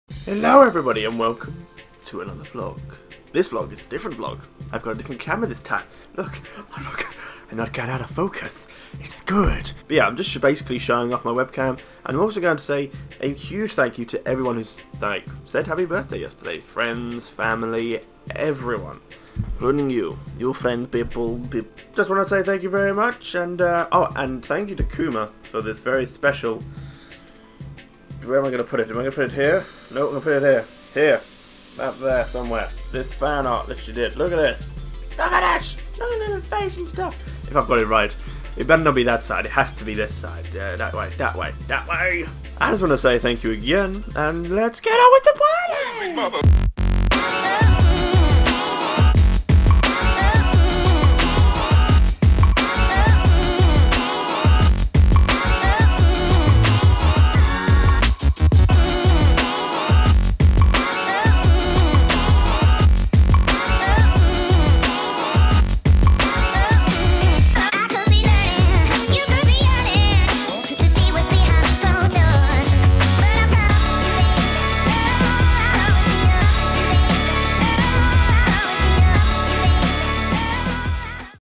THANKYOU!!
Just a happy lickle vlog to say thankyou! (WARNING: May contain epic dance moves)
So who here likes banter?